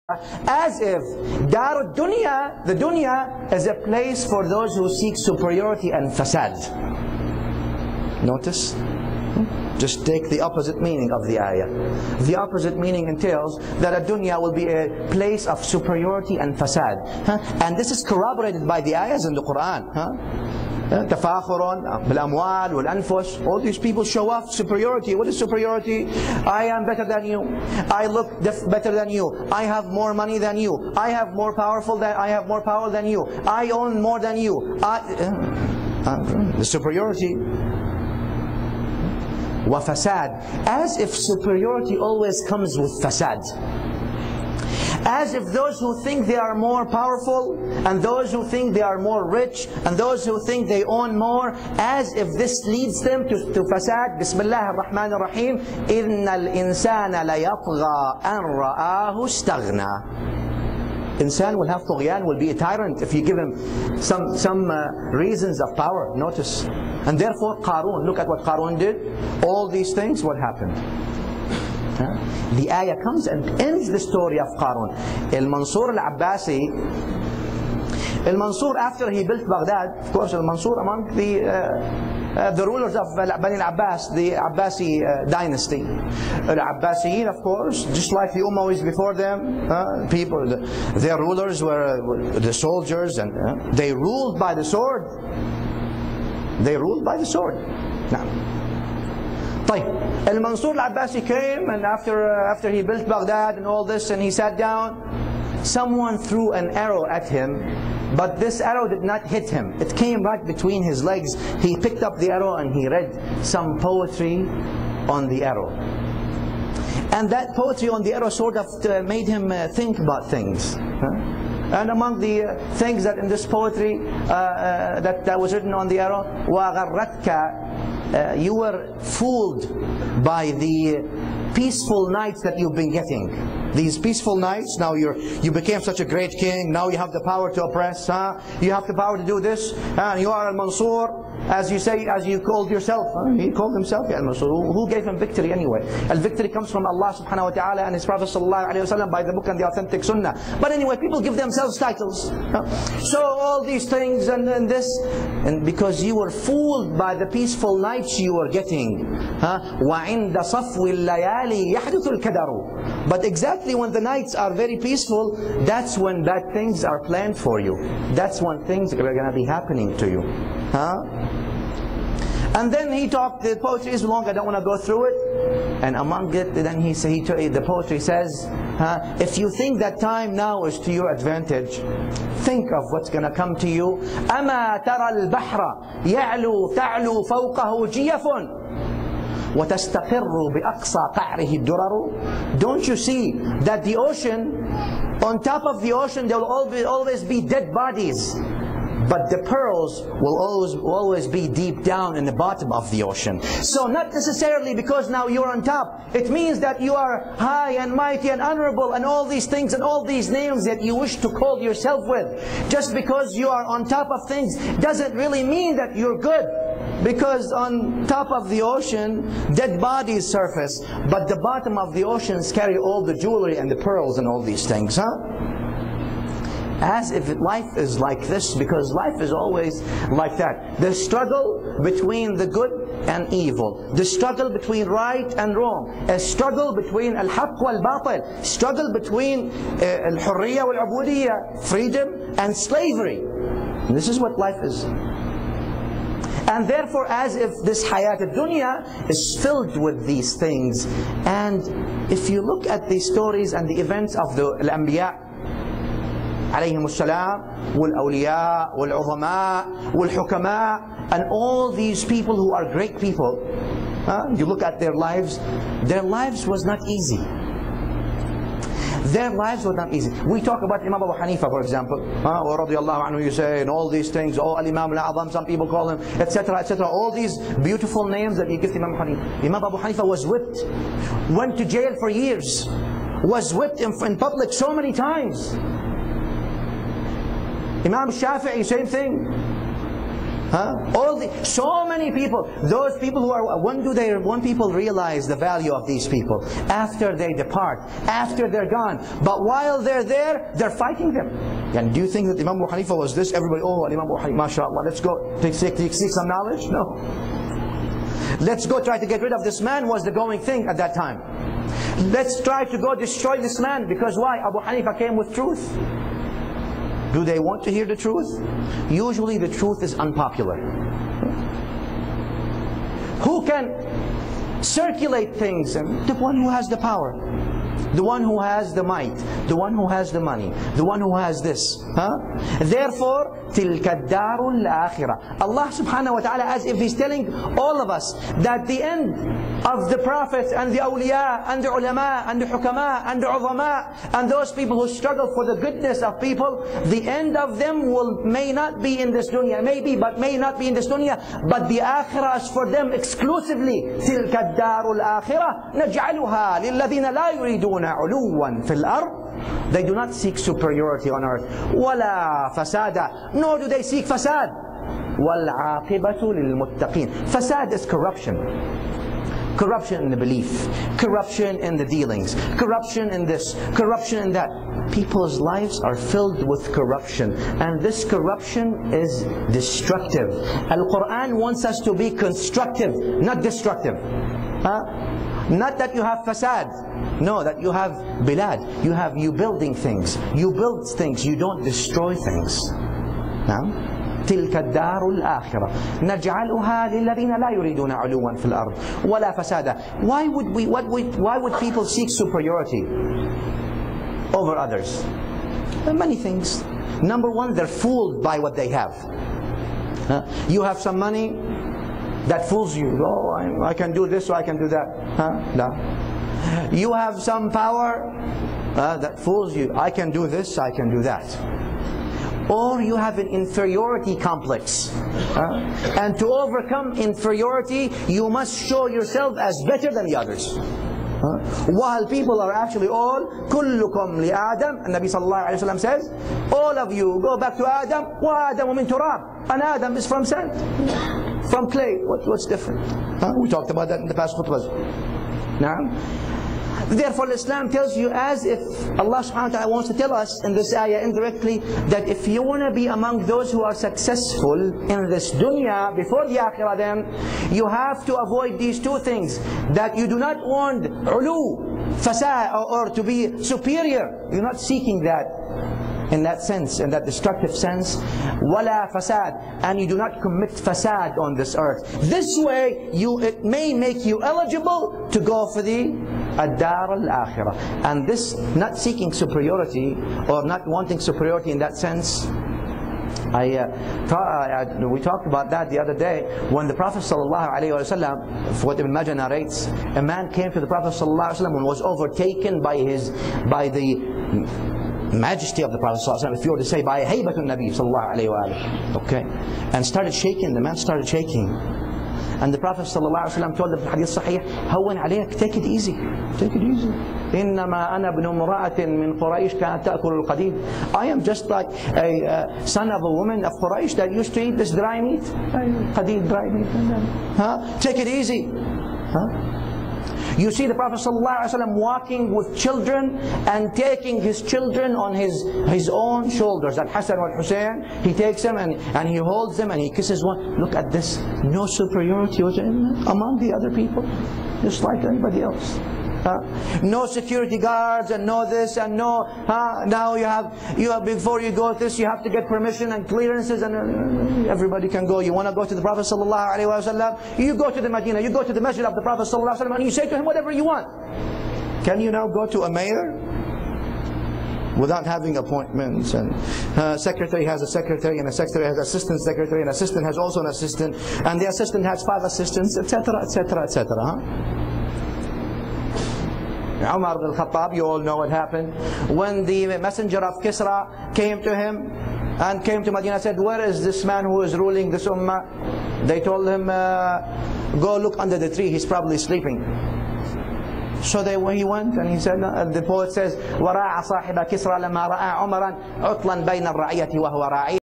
Khutbah 95 - To Whom Belongs The Hereafter (3/4) By Shaykh Ninowy
An inspiring Lecture by Allama Shaykh Dr. Muhammad bin Yahya Al Husayni Al Ninowy (Hafithahu Allah).